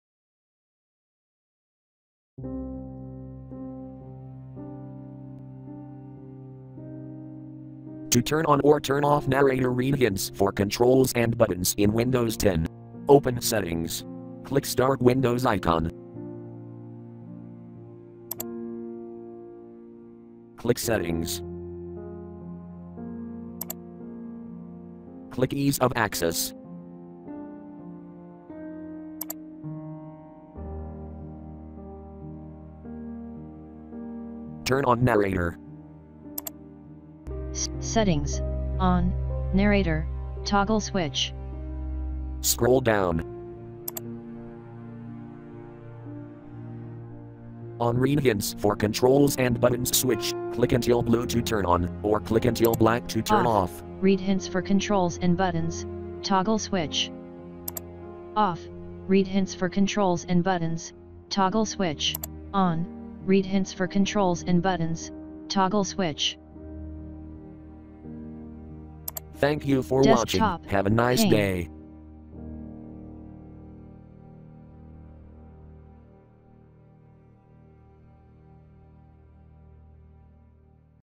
Windows 10 : How to turn on or turn off narrator read hints for controls and buttons
This video show How to turn on or turn off narrator read hints for controls and buttons in Windows 10 Pro. I use Dell Inspiron 14 3000 Series in this tutorial